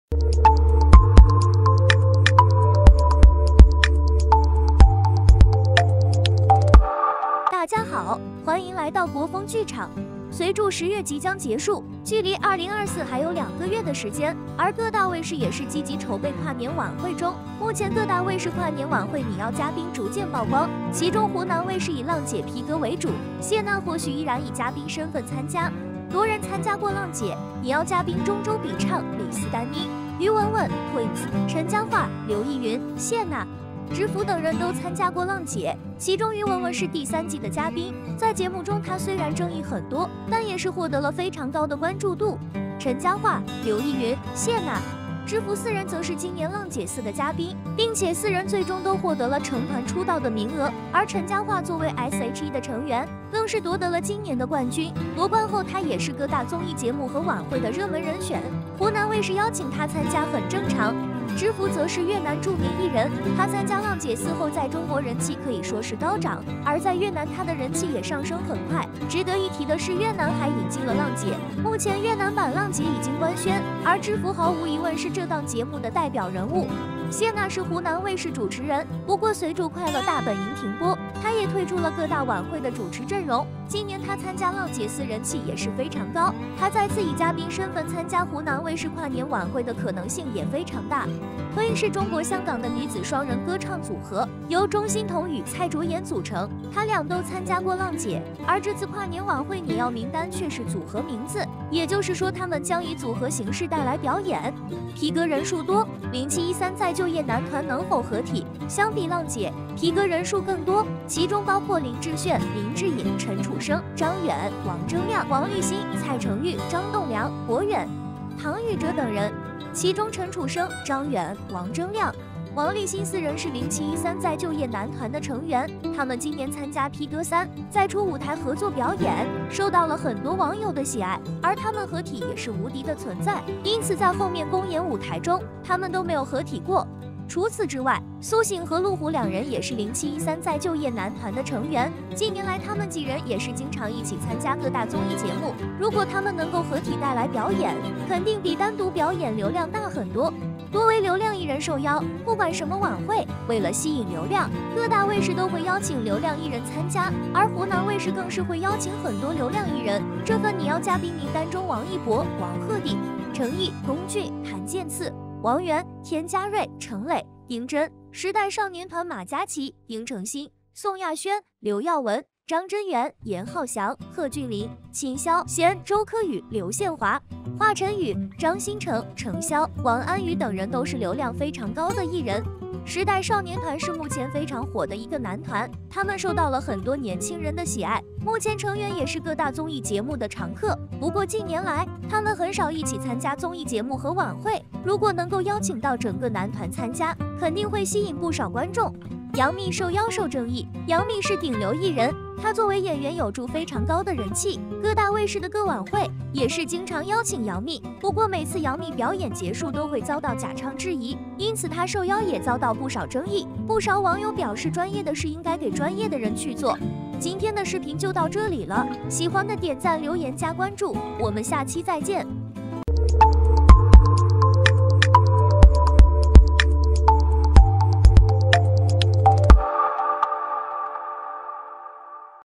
湖南跨年晚會嘉賓曝光，以《浪姐》《披哥》班底為主，王心凌再度缺席引爭議！
湖南跨年晚會嘉賓曝光，以《浪姐》《披哥》班底為主，王心凌再度缺席引爭議！王心凌